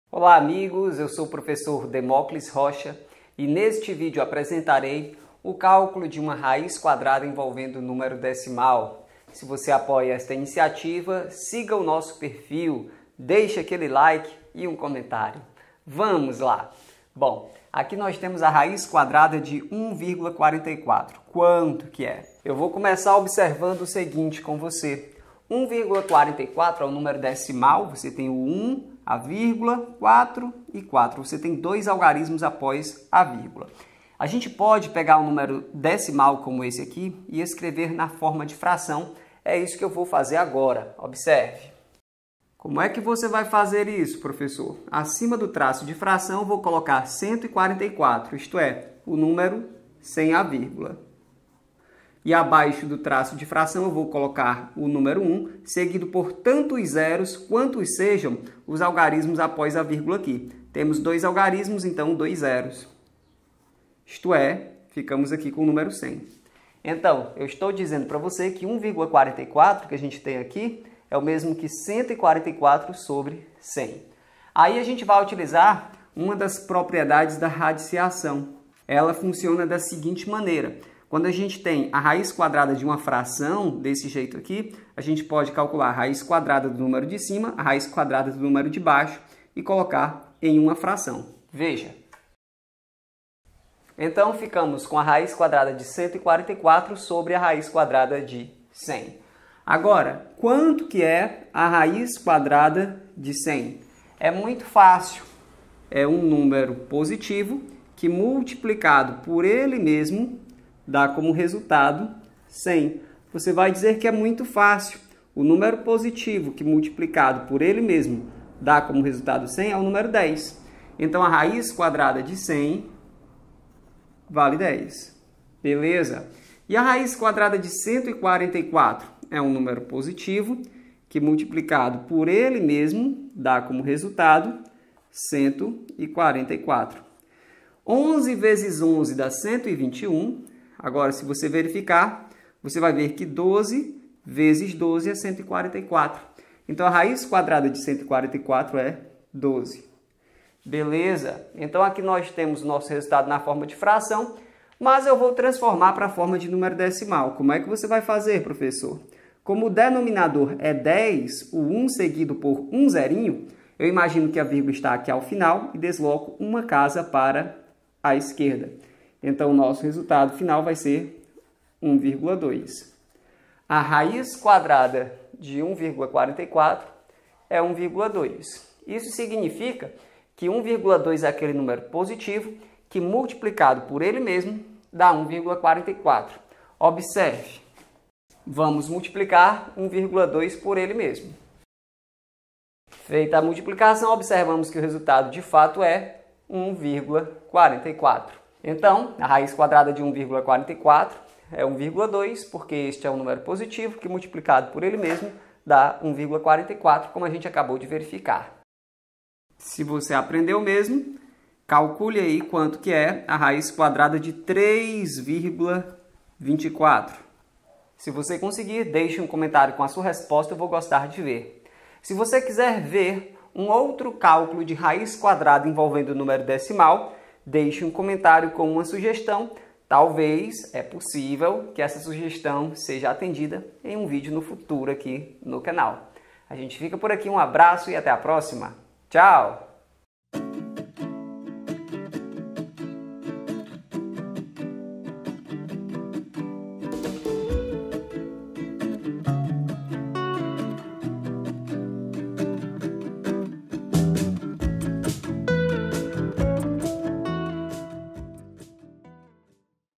RAIZ QUADRADA
Olá, amigos do meu canal no YouTube. Eu sou o professor Demóclis Rocha e, neste vídeo, apresentarei uma forma de calcular a raiz quadrada de um número decimal.

CÂMERAS

MICROFONES